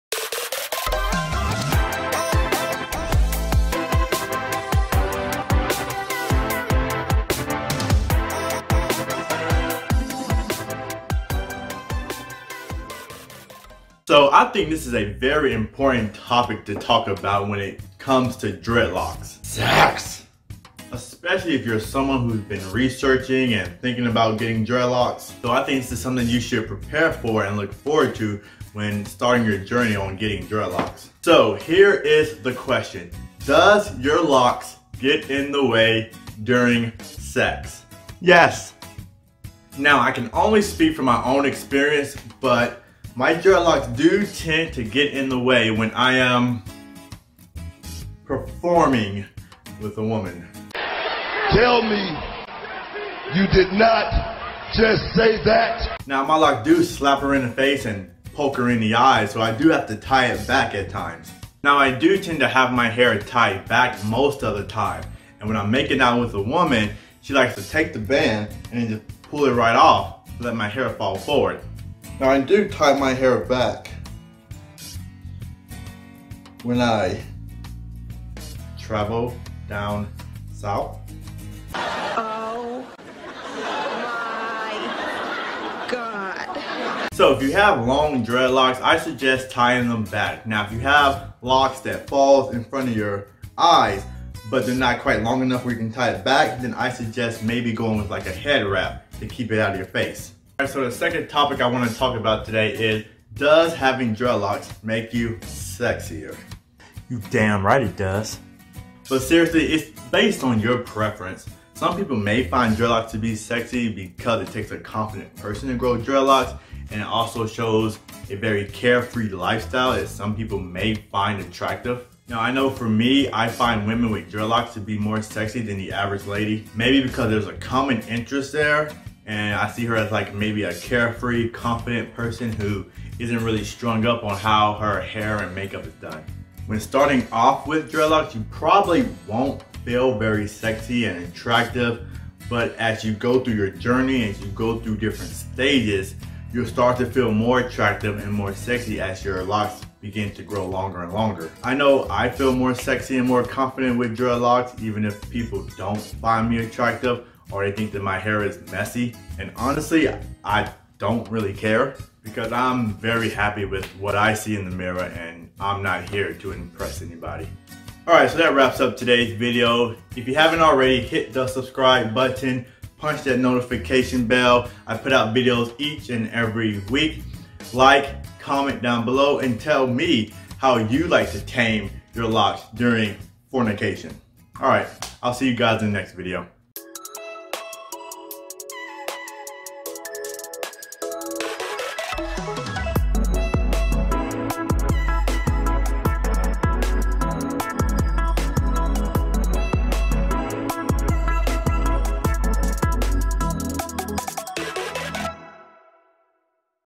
SEX & DREADlocs |Does dreads make you sexier
In todays video, I am going to talk about having sex with  dreadlocs and how to keep them from getting in the  way. Second topic I am going to talk about is. Does having dreadlocs make you sexier. Does dreadlocs  attract other people. I dive into these two topics today .